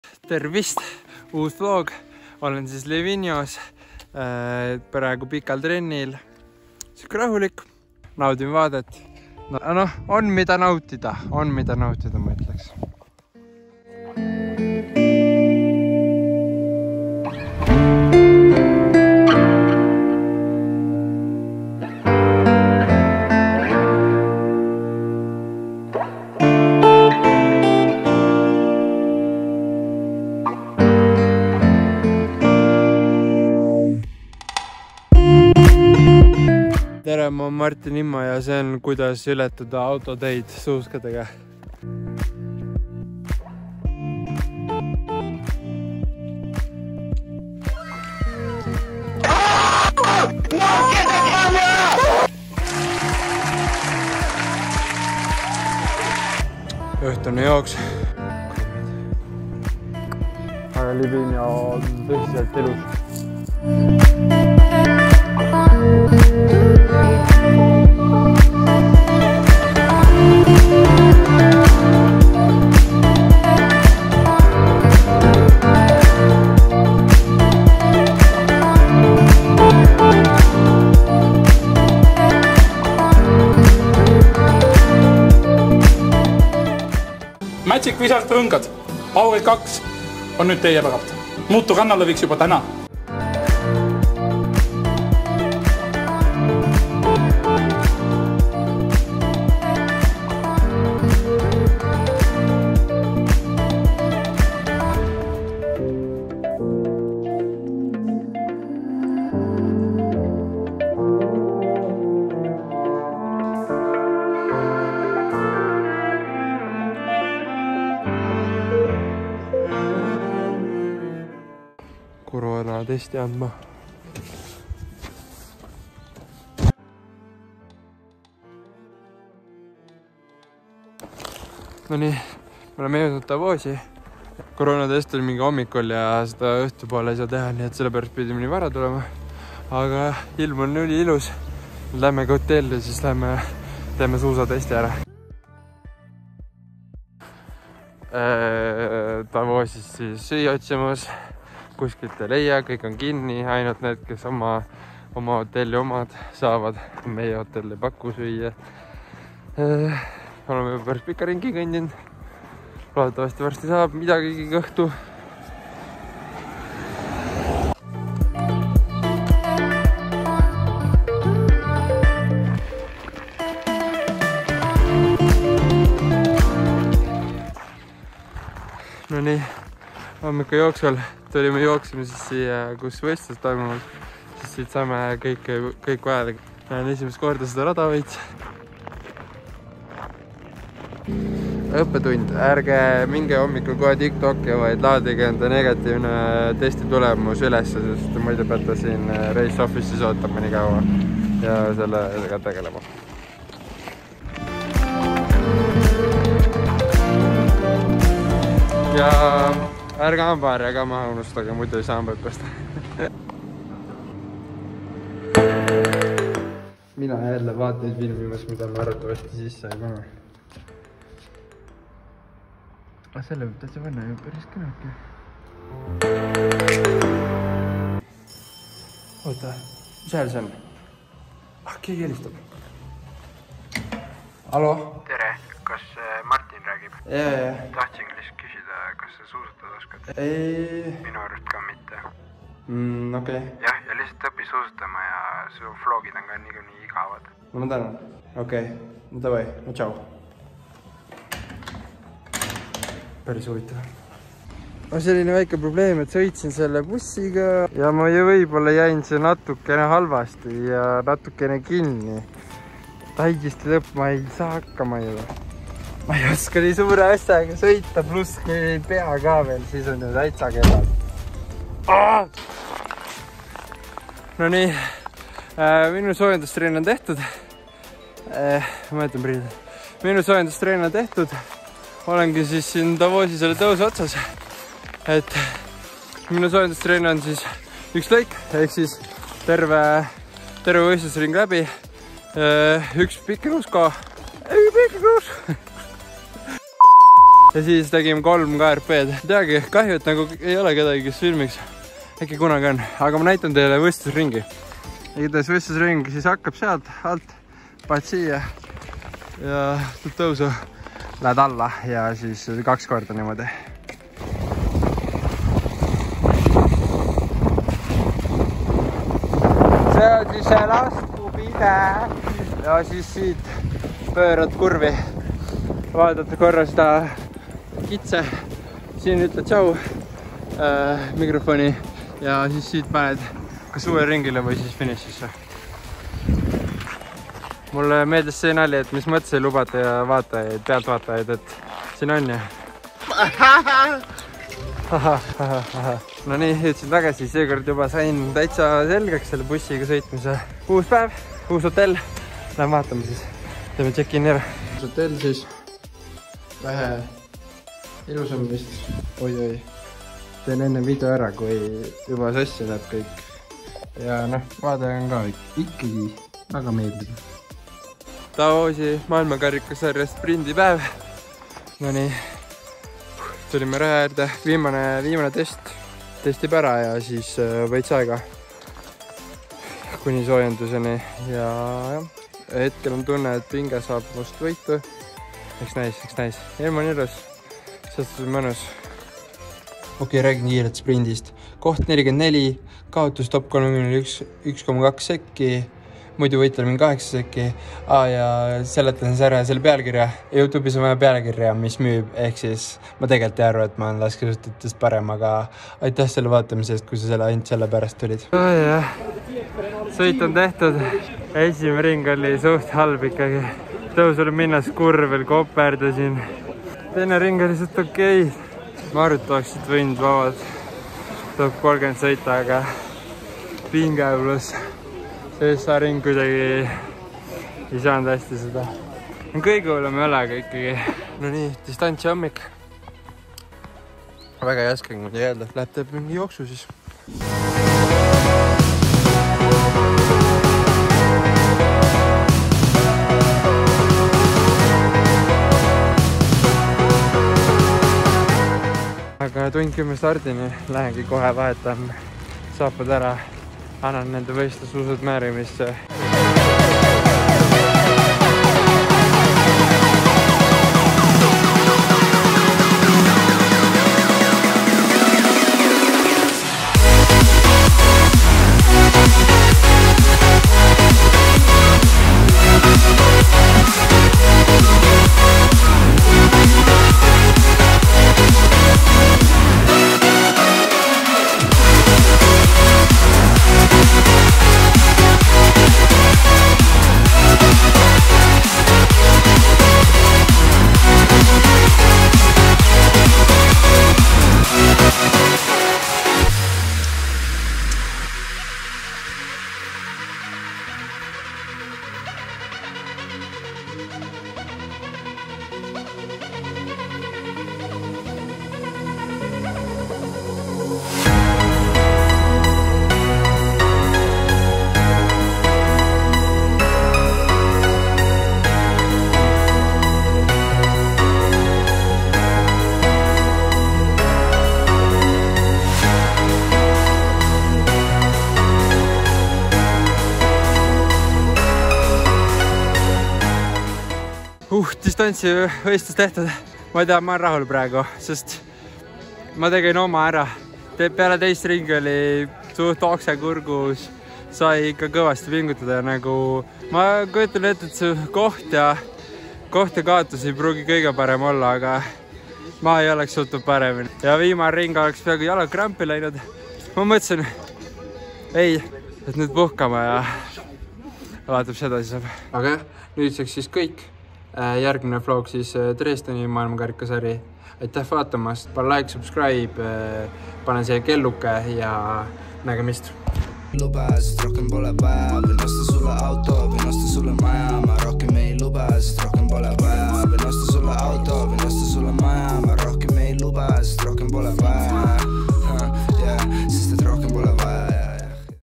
TÕESTUS, ET MA OLEN PAREM KUI LASKESUUSATAJAD
Davosi maailmakarikasarja vlog. aaa ja ärge mind süüdistage, et youtube nii töötab😉 (see pealkiri ja pisipilt)